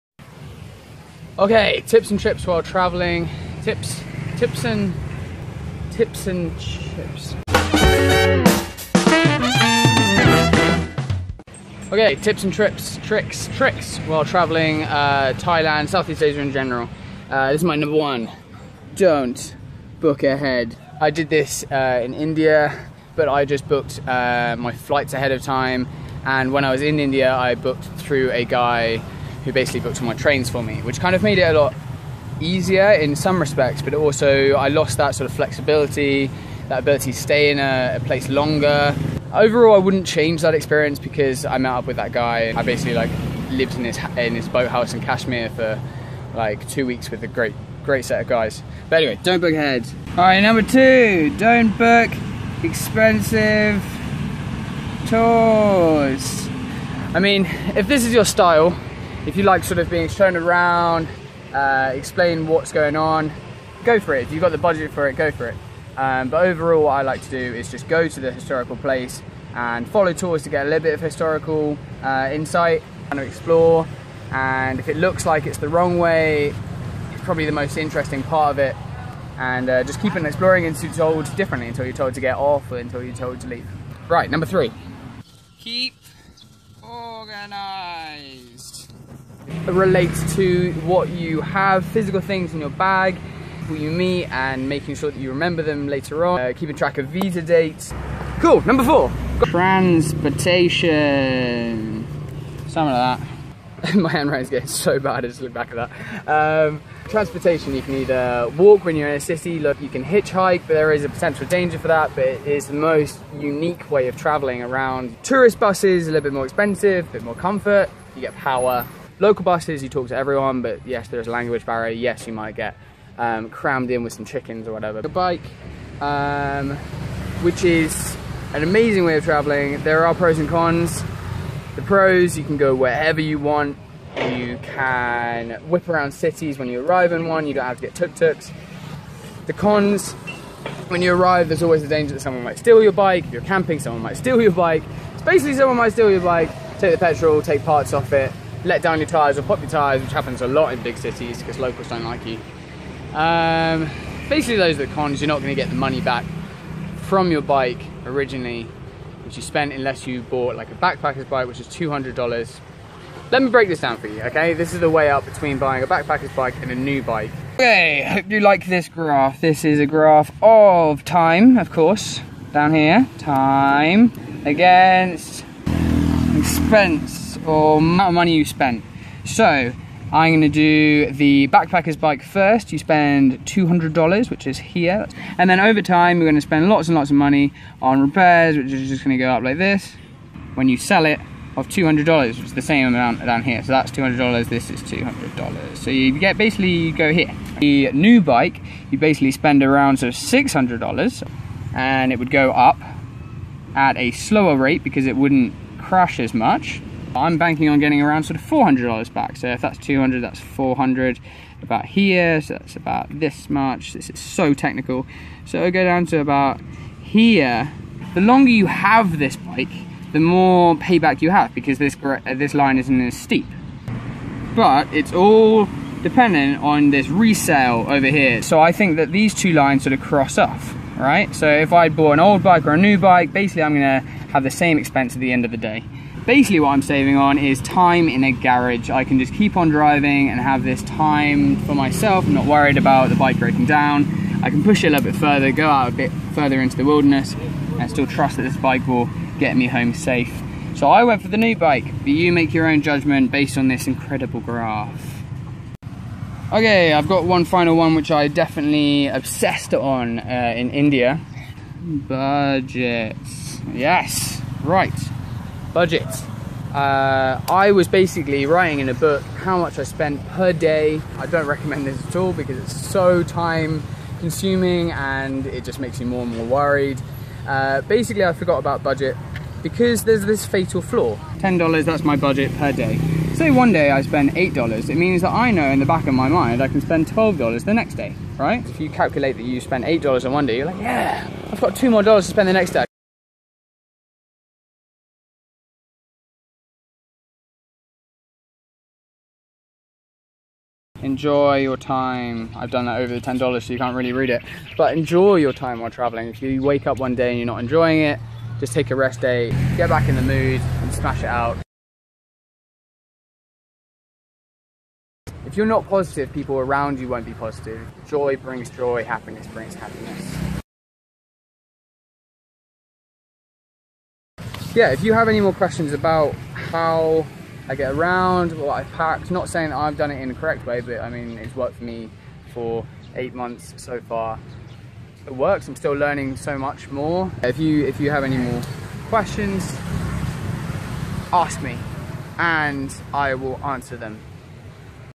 How I travel
Traveling is all about finding a unique experience for yourself.
Before I left to go traveling I watched lots of videos and asked all my traveling friends advice on how to do it.

I'll break it down.

Before traveling:
Do basic research of climate, attractions, costs...
but most important ESCAPE ROUTES. Spend a bit of time planning what happens if...

While traveling:
Just relax, take your time, never rush
Talk to people and learn.
Time is the only thing which is yours. use it before you loose it